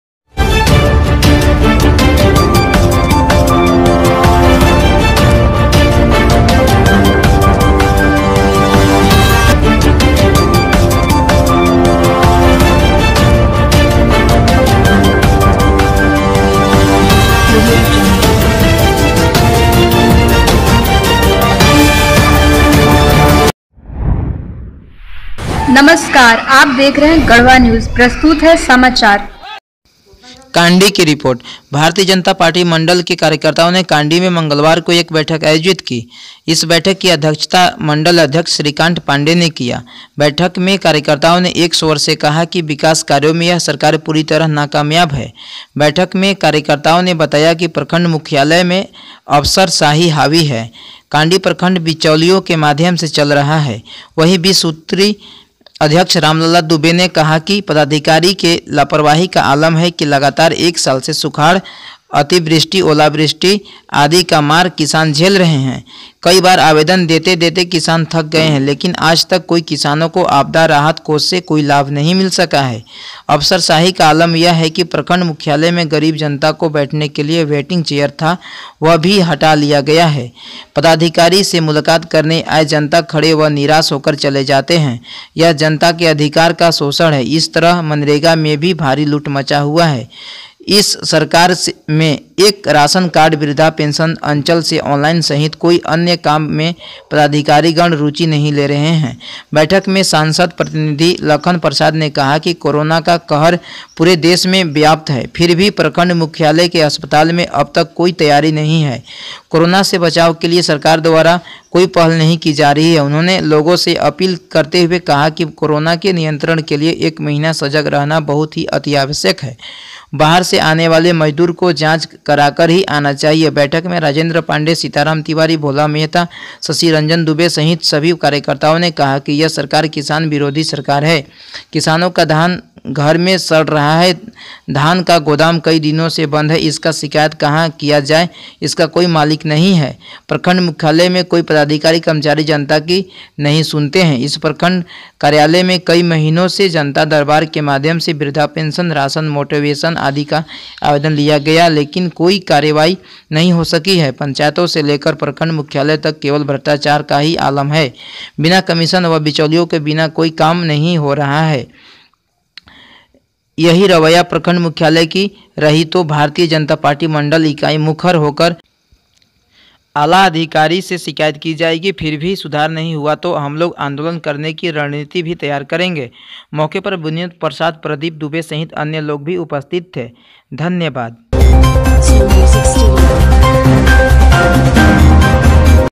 भारतीय जनता पार्टी की बैठक// Ss Garhwa news tv
गढ़वा : भारतीय जनता पार्टी मंडल के कार्यकर्ताओं ने कांडी में मंगलवार को एक बैठक आयोजित की। इस बैठक की अध्यक्षता- मंडल अध्यक्ष श्रीकांत पांडेय ने की। बैठक में कार्यकर्ताओं ने एक स्वर से कहा कि विकास कार्यों में यह सरकार पूरी तरह नाकामयाब है। बैठक में कार्यकर्ता ने बताया कि प्रखंड मुख्यालय में अफसरशाही हावी है। कांडी प्रखंड बिचौलियों के माध्यम से चल रहा है। वहीं पूर्व बीस सूत्री अध्यक्ष- रामलला दुबे ने कहा कि पदाधिकारी के लापरवाही का आलम है की लगातार 1 साल से सुखाड़, अतिवृष्टि,ओलावृष्टि आदि का मार किसान झेल रहे हैं। कई बार आवेदन देते-देते किसान थक गए, लेकिन आज तक कोई किसानों को आपदा राहत कोष से कोई लाभ नहीं मिल सका। अफसरशाही का आलम यह कि प्रखंड मुख्यालय में गरीब जनता को बैठने के लिए वेटिंग चेयर था, वह भी हटा लिया गया। पदाधिकारी से मुलाकात करने आए जनता खड़े व निराश होकर चले जाते हैं। यह जनता के अधिकार का शोषण है। इसी तरह मनरेगा में भी भारी लूट मचा हुआ है। इस सरकार में एक राशन कार्ड,वृद्धा पेंशन,अंचल से ऑनलाइन सहित कोई अन्य काम मे पदाधिकारीगण रुचि नहीं ले रहे हैं। बैठक में सांसद प्रतिनिधि- लखन प्रसाद ने कहा कि कोरोना का कहर पूरे देश में व्याप्त है, फिर भी प्रखण्ड मुख्यालय के अस्पताल में अब तक कोई तैयारी नहीं। कोरोना से बचाव के लिए सरकार द्वारा कोई पहल नहीं कि जा रही है। उन्होंने लोगों से अपील करते हुए कहा कि कोरोना के नियंत्रण के लिए एक महीना सजग रहना बहुत ही आवश्यक है। बाहर से आने वाले मजदूर को जांच कराकर ही आना चाहिए। बैठक में राजेंद्र पांडेय,सीताराम तिवारी,भोला मेहता,शशि रंजन दुबे सहित सभी कार्यकर्ताओं ने कहा कि यह सरकार किसान विरोधी सरकार है। किसानों का धान घर में सड़ रहा है। धान का गोदाम कई दिनों से बंद है। इसका शिकायत कहां किया जाए, इसका कोई मालिक नहीं है। प्रखंड मुख्यालय में कोई पदाधिकारी कर्मचारी जनता की नहीं सुनते। इस प्रखंड कार्यालय में कई महीनों से जनता दरबार के माध्यम से वृद्धा पेंशन,राशन, मोटेशन आदि का आवेदन लिया गया,लेकिन कोई कार्रवाई नहीं हो सकी। पंचायतों से लेकर प्रखंड मुख्यालय तक केवल भ्रष्टाचार का ही आलम है। बिना कमीशन व बिचौलिए के बिना कोई काम नहीं हो रहा है। जनता अत्यंत त्रस्त है। बैठक में लोगों ने कहा कि यही रवैया प्रखण्ड मुख्यालय की रही तो भारतीय जनता पार्टी मंडल इकाई मुखर होकर आला अधिकारी से शिकायत की जाएगी। फिर भी सुधार नहीं हुआ तो हमलोग आंदोलन करने की रणनीति भी तैयार करेंगे। मौके पर- विनोद प्रसाद,प्रदीप दुबे सहित अन्य लोग भी उपस्थित थे।